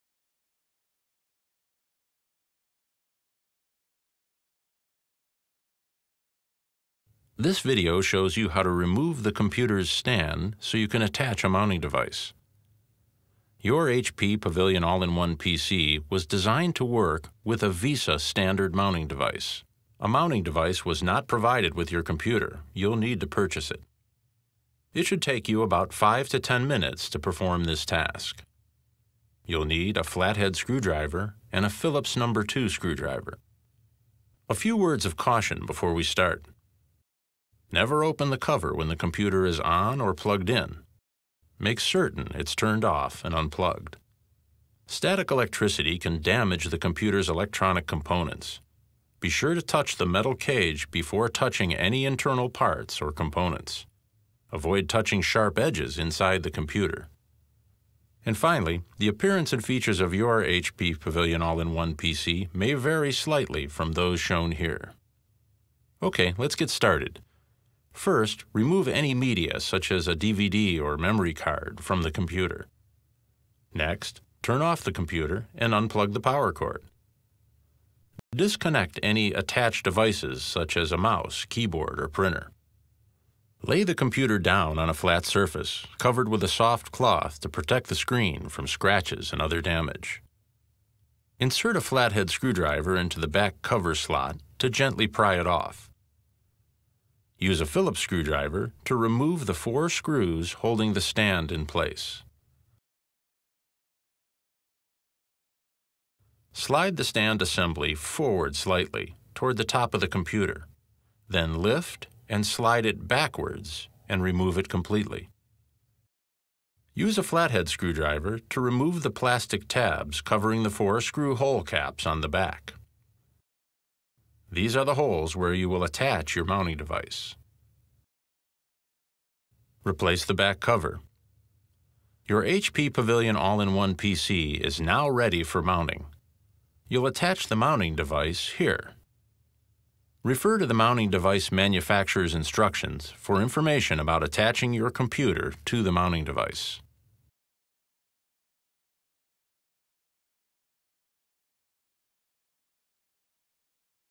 Removing the Monitor Stand for Wall Mounting | HP Pavilion All-in-One PC
Learn how to remove your computer monitor's stand so you can attach a wall mounting device.